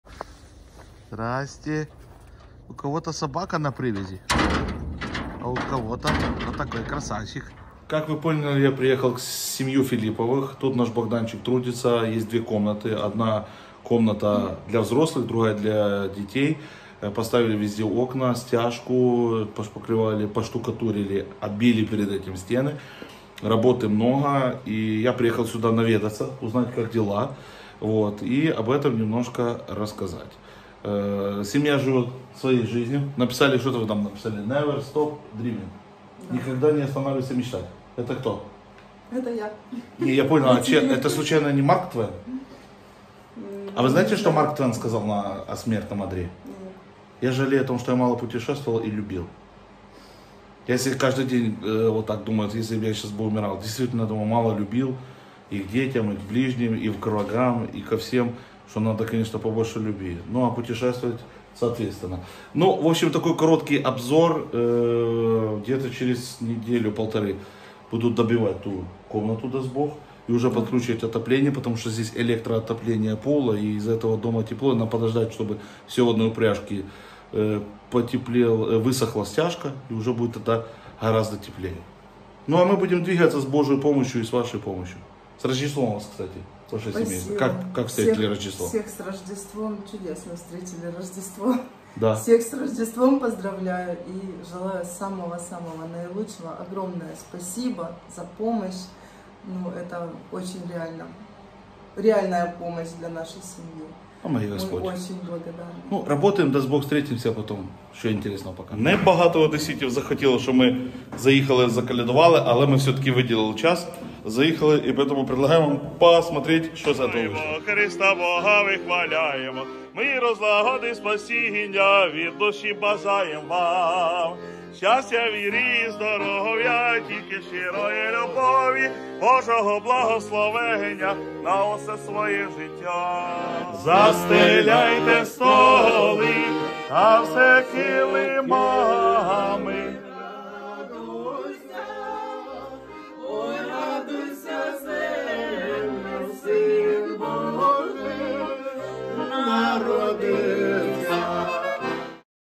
Радость в каждый дом
Помогли семье Филипповых на ремонт дома 67000
Помогать людям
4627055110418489